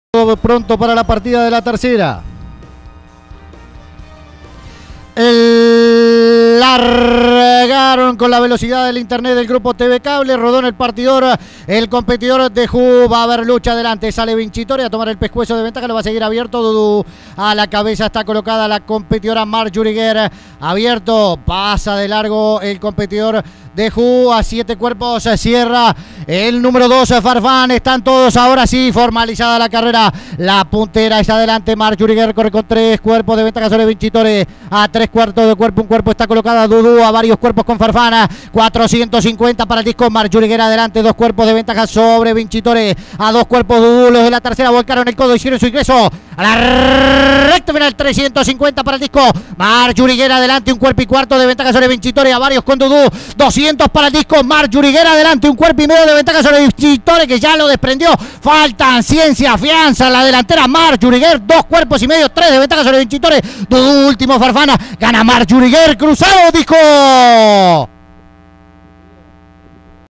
2015-273 Hipódromo Miguel Salem - Marjorie Girl (USA) sep. 13, 2015
MARJORIE GIRL (USA-2010) hembra castaña por Whywhywhy y Grand Angel (Grand Slam). Tercera victoria en 13 salidas. Jinete: Joffre Mora (Ecu); Preparador: Hernando Díaz (Col); Stud: David y Daniel. Distancia 1.000 metros. Tiempo: 1’00”3 (pista semipesada).
1º) Marjorie Girl (USA), 2º) Vincitore (USA), 3º) Dudú (Arg). Todos corrieron.